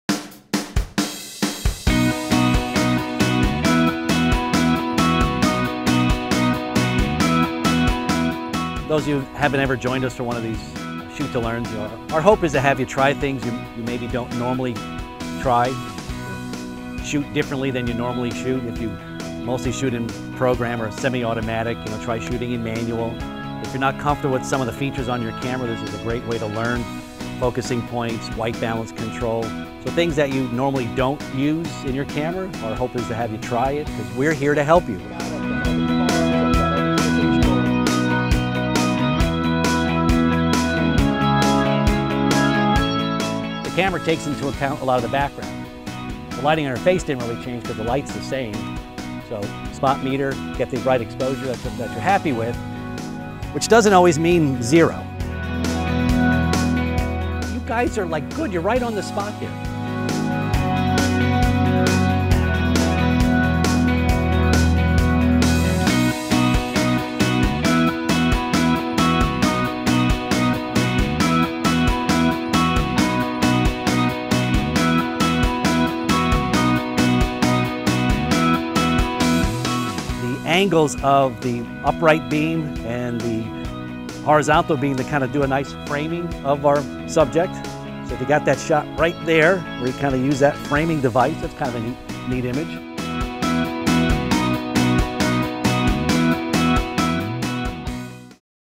Shoot to Learn: Portraits at the Bridge with Dan's Camera City
This May, our instructor TJ led a group of students in this fun on-location workshop, where they learned about outdoor portrait photography & working with a model in a scenic location
at Kreidersville Covered Bridge in Allen Township, PA.